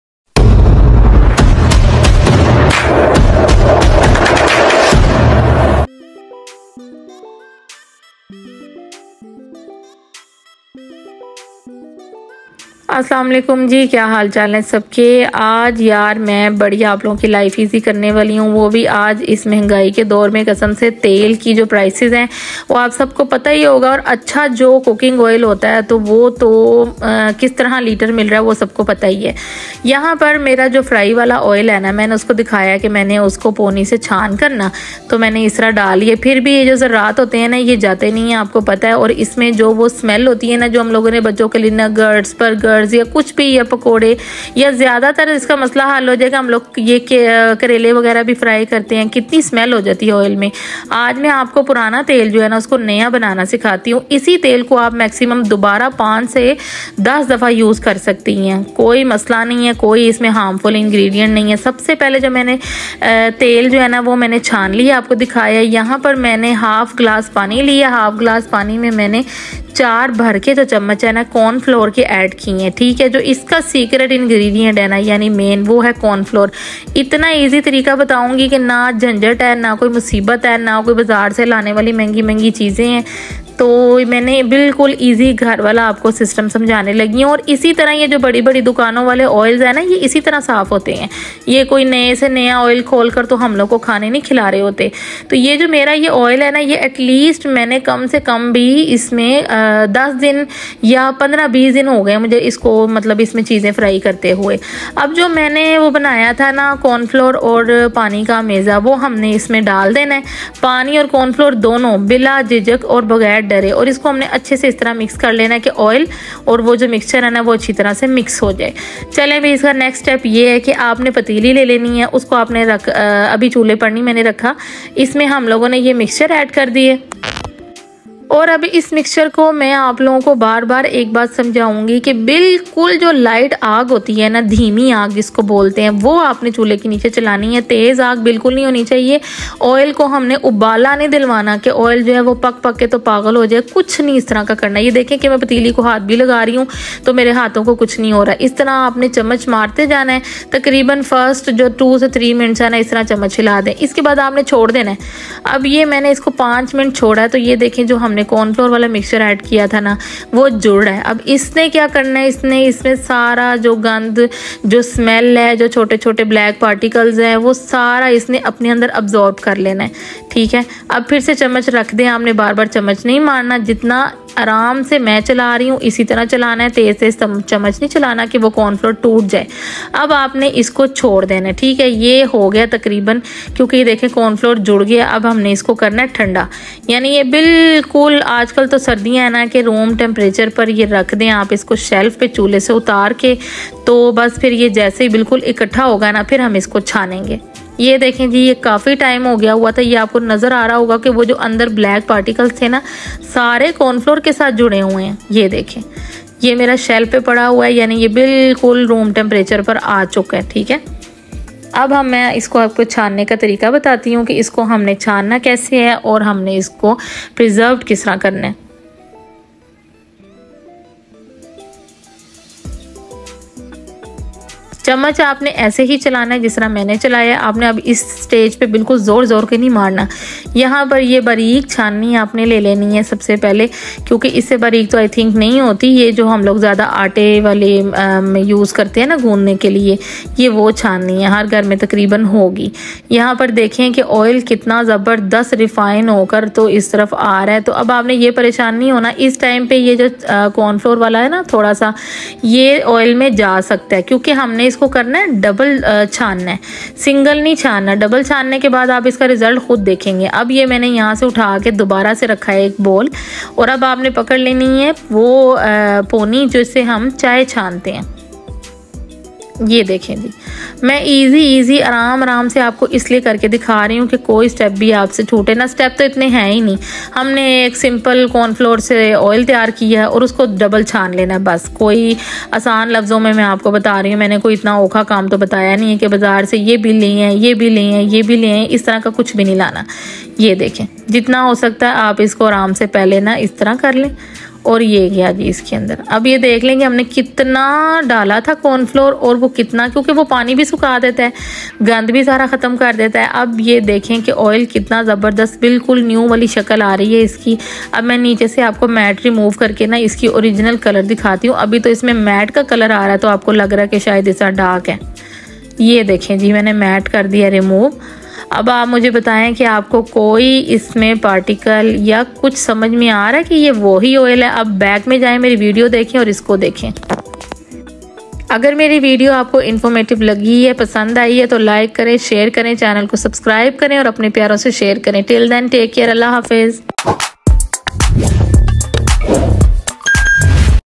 How To Clean Dirty Black Cooking Oil - Trick To Clean And Reuse Frying Oil
Today i am sharing a great trick or hack of oil reusing
In this video i am sharing how to filter cooking oil in home
And we can again use this as new oil